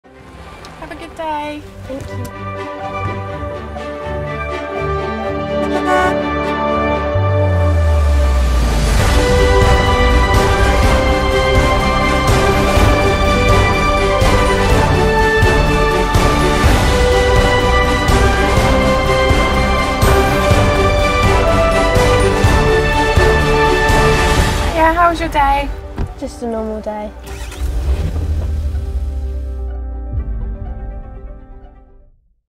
Trailer for Millie's Day at Worthing High School
As part of our Virtual Open Evening 2020 event, we will be sharing a video with all prospective parents, carers and students which documents a day in the life of "Millie" at Worthing High. The video will be released next week - here is the trailer! Keeping checking our twitter and YouTube Channel for the full length version.